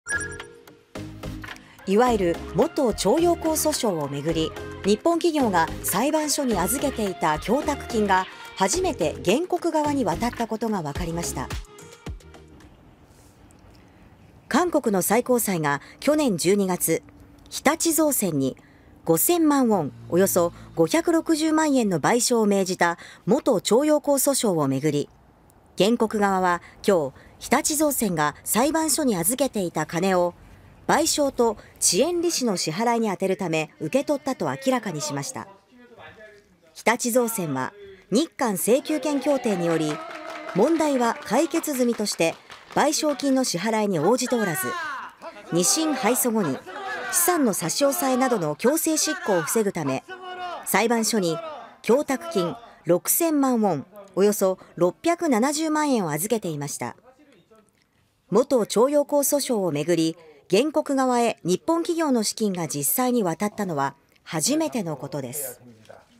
元徴用工訴訟巡り日本企業の供託金が原告側へ 韓国(2024年2月20日)
いわゆる「元徴用工訴訟」を巡り、日本企業が裁判所に預けていた供託金が初めて原告側に渡ったことが分かりました。

　韓国の最高裁が去年12月、「日立造船」に5000万ウォン、約560万円の賠償を命じた元徴用工訴訟を巡り、原告側は今月20日、日立造船が裁判所に預けていた金を賠償と遅延利子の支払いに充てるため受け取ったと明らかにしました。

　日立造船は日韓請求権協定により、「問題は解決済み」として賠償金の支払いに応じておらず、2審敗訴後に資産の差し押さえなどの強制執行を防ぐため、裁判所に供託金6000万ウォン、約670万円を預けていました。

　元徴用工訴訟を巡り、原告側へ日本企業の資金が実際に渡ったのは初めてのことです。